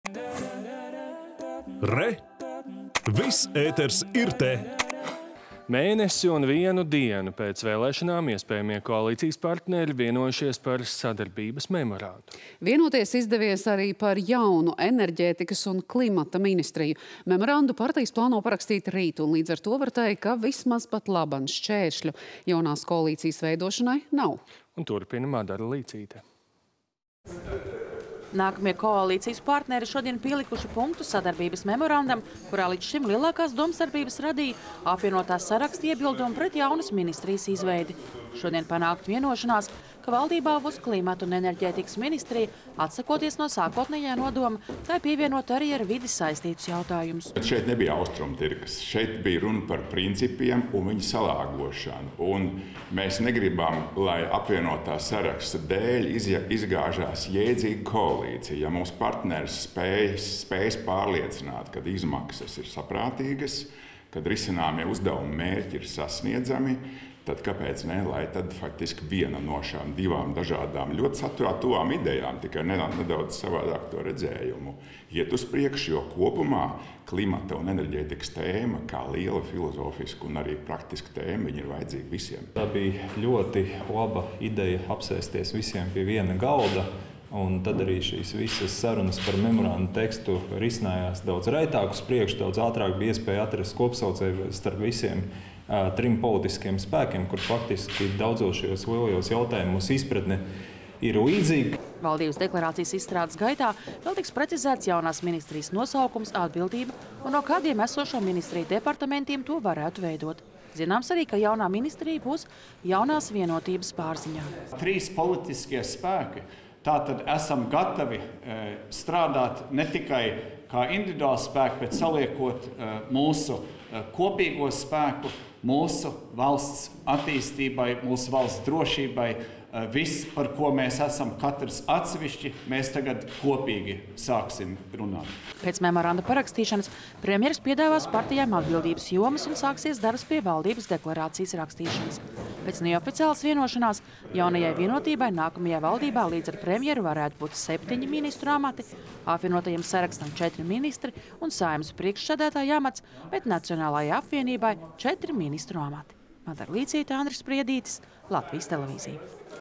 Partijas panāk vienošanos par sadarbības memorandu
Mēnesi un vienu dienu pēc vēlēšanām iespējamie koalīcijas partneri vienojušies par sadarbības memorandu. Vienoties izdevies arī par jaunu – Enerģētikas un klimata ministriju. Memorandu partijas plāno parakstīt rīt, un līdz ar to var teikt, ka vismaz patlaban šķēršļu jaunās koalīcijas veidošanai nav. Turpina Madara Līcīte.